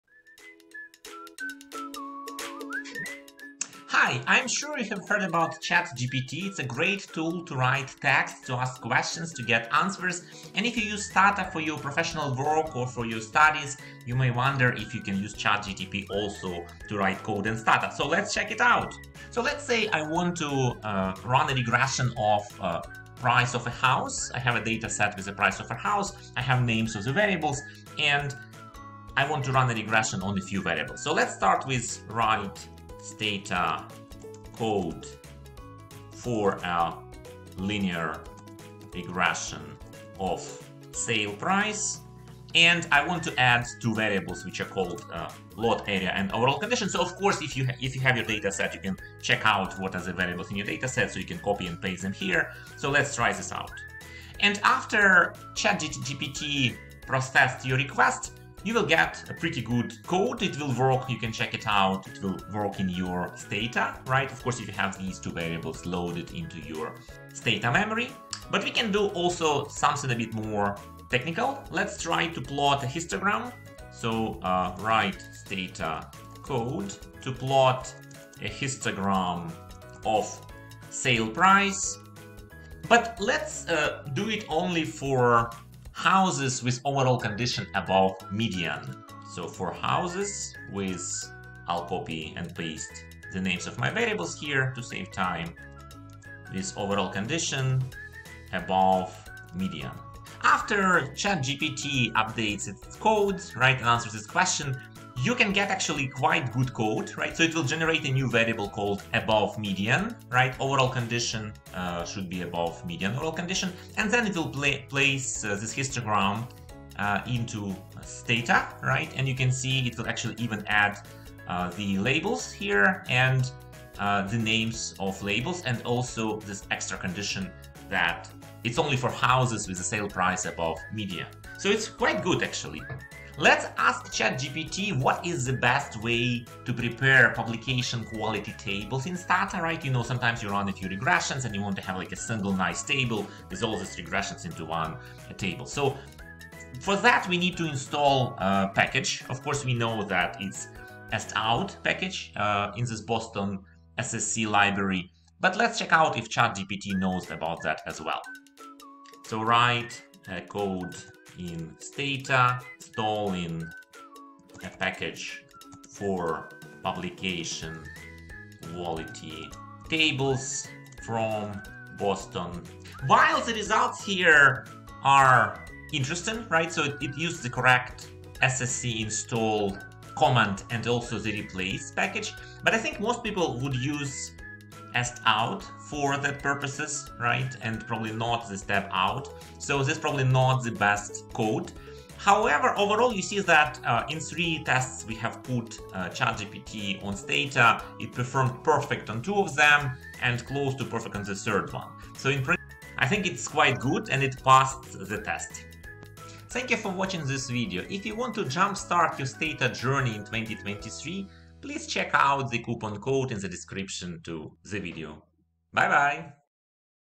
Unlock the Power of Stata with ChatGPT!
if this video saved you some time ☕️

In this video, we analyze how good ChatGPT is at writing code for stata using 3 examples. ChatGPT could write a simple code to run linear regression, plot a histogram for the sales price of houses with overall condition above the median (the hard one!), and install a package from SSC Boston (somewhat mixed result).

⏱️VIDEO CHAPTERS⏱️
0:00 Introduction to Stata Coding with ChatGPT
0:30 Stata Code to Run a Linear Regression
1:10 Stata Code to Plot a Histogram With IF Condition
2:15 Stata Code to Prepare Publication Quality Table